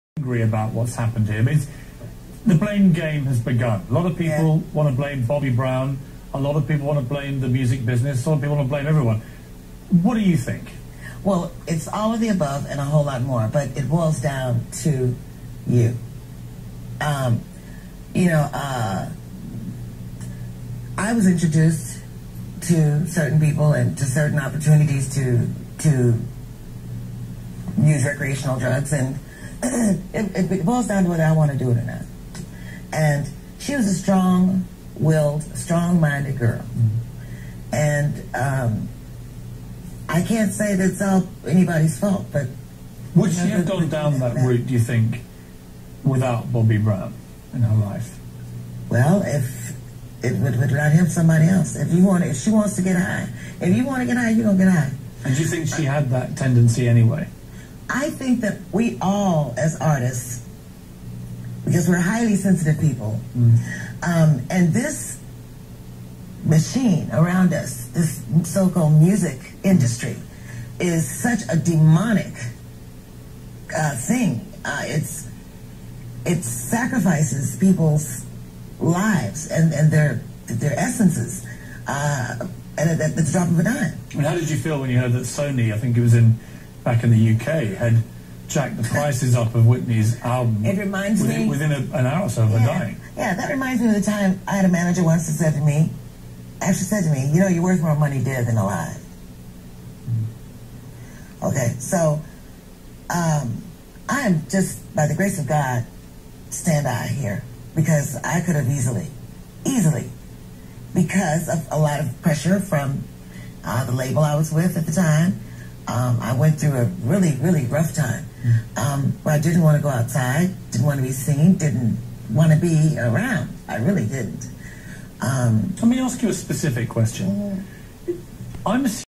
Chaka Khan ⚡️THE MACHINE AROUND US IS DEMONIC ⚡️ Feb 14 2012
Chaka Khan opens up on CNN to reveal her true feelings about who she sold her soul to, on the topic of Whitney Houston's death and what may have caused her demise.  Scrubbed from CNN!!